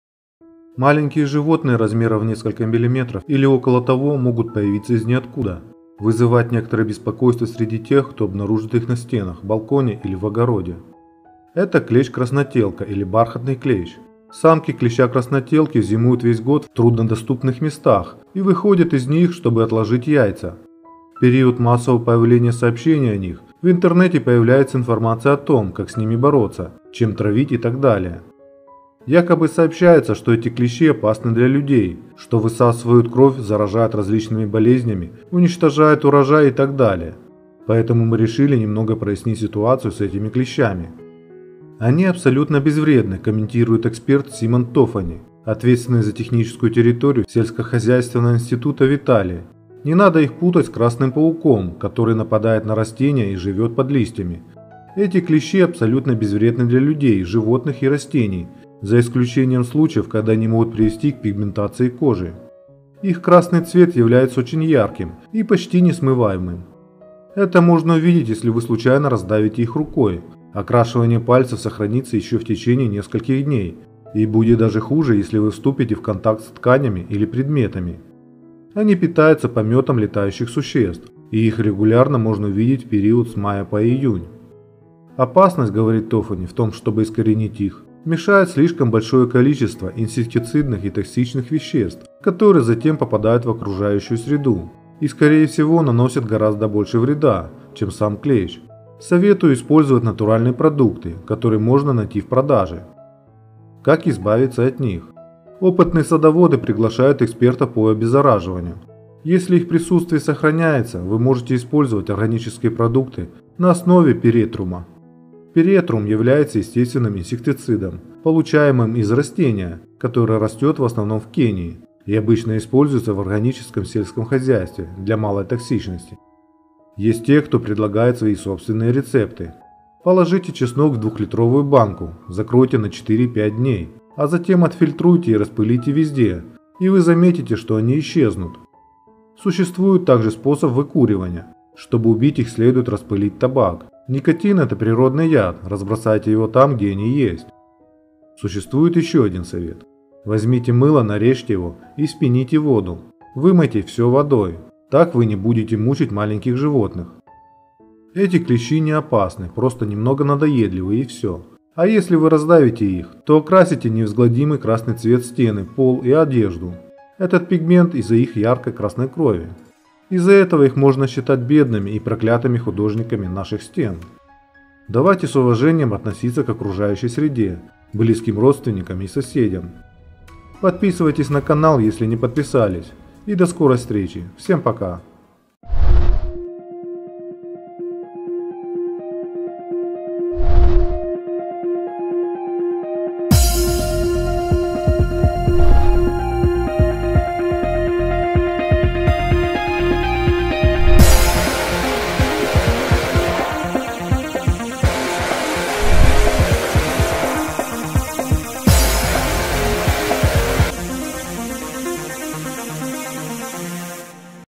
Чем опасны красные клещи-краснотелки при нападении
0:00 - Клещ-краснотелка или бархатный клещ
1:42 - Как избавиться от клеща-краснотелки